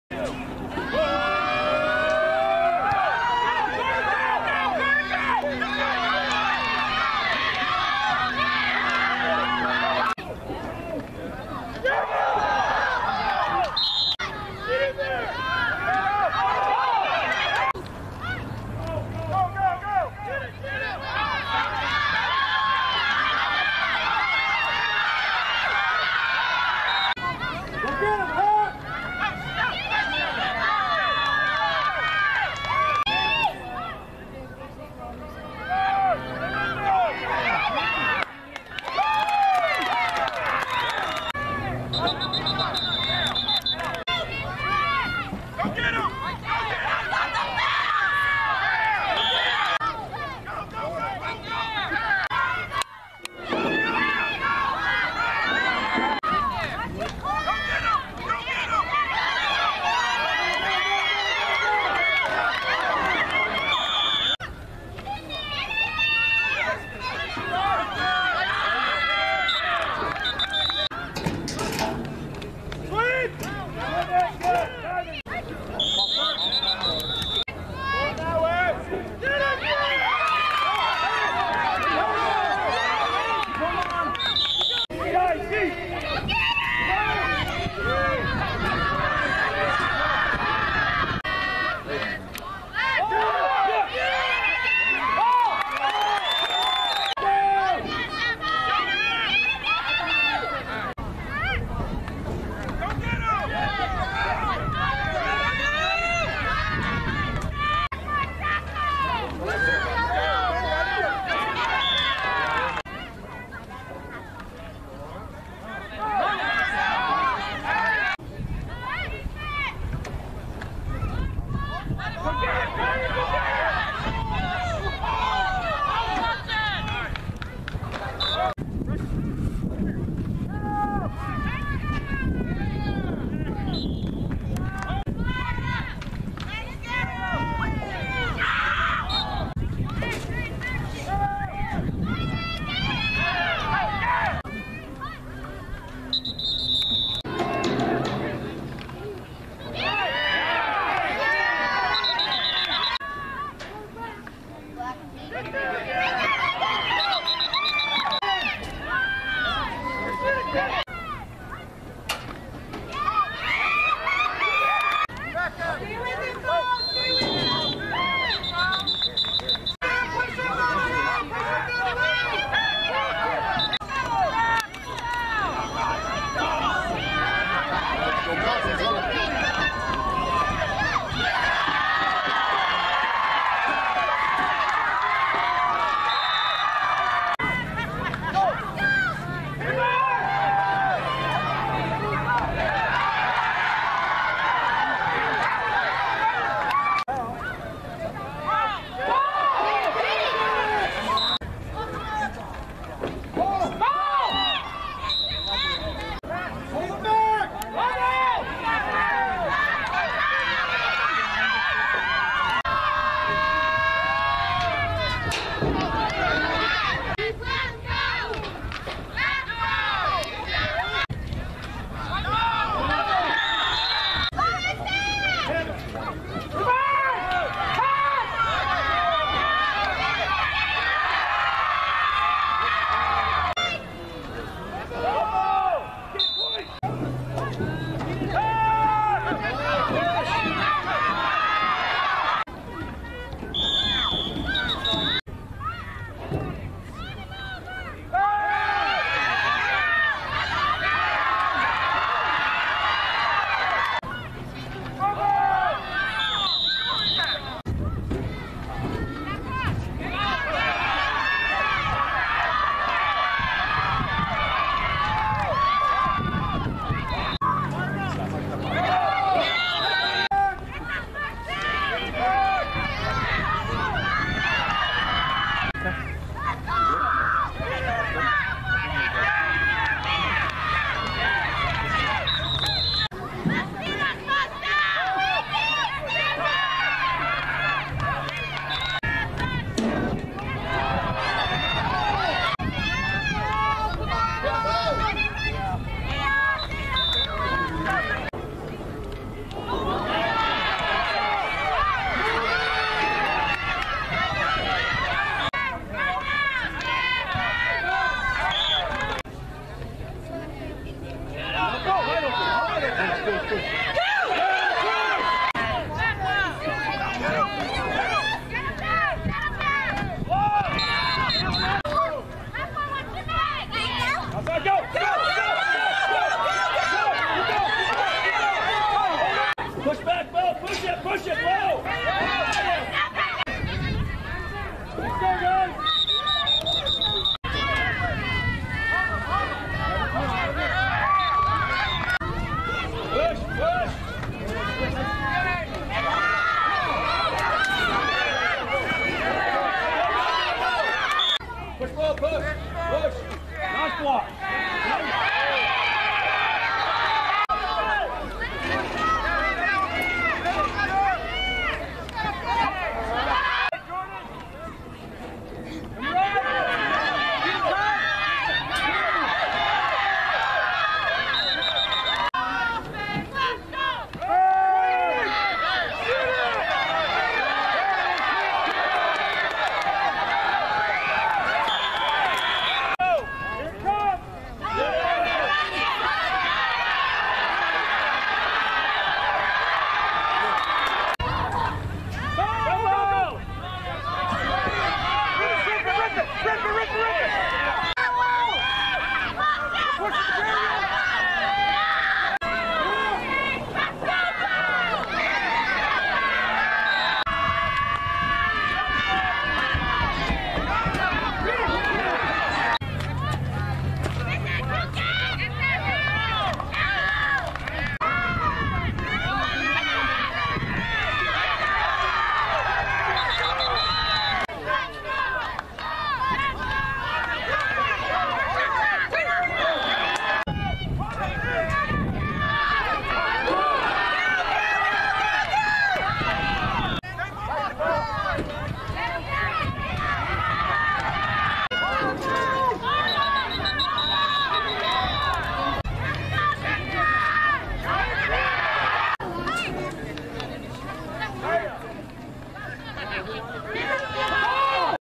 Jr Pee Wee- Plain City vs Ogden Wildcats 08-22-20
Jr Pee Wee Plain City vs Ogden Wildcats
08-22-20
Final Score: Plain City: 0    Ogden Wildcats: 8